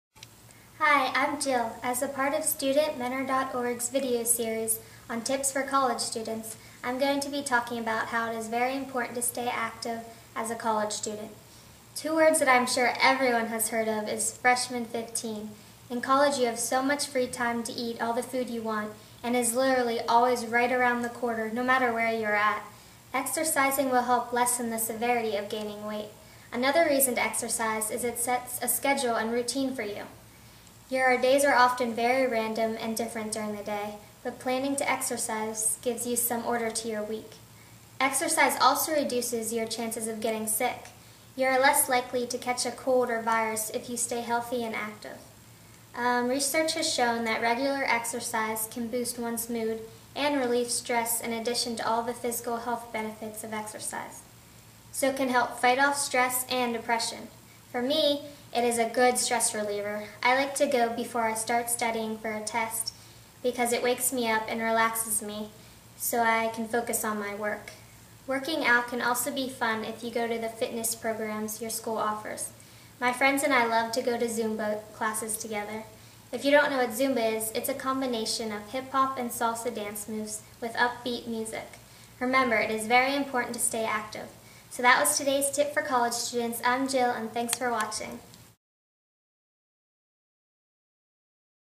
"Staying Physically Active: Tips for College Students" StudentMentor.org's Student Video Blog Series
College student Jill shares her tips for staying physically active.